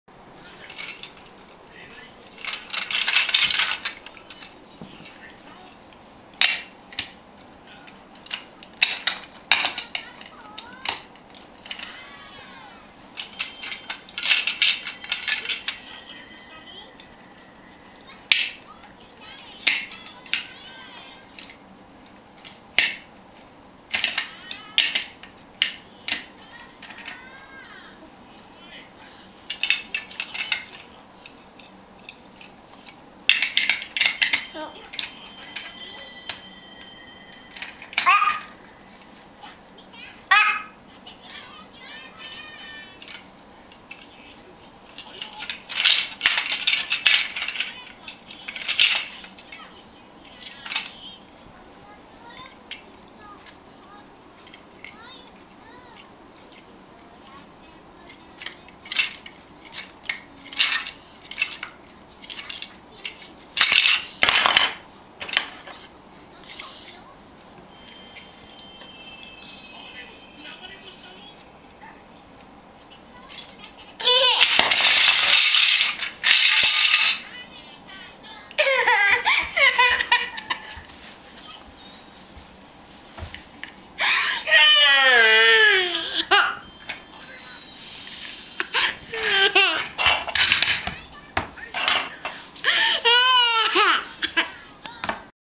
レゴで遊ぶけど、、上手にできなくて泣くT-T
せんのすけ、楽しそうに遊んでましたが、、、
上手にできなくて・・・泣いちゃった(^-^;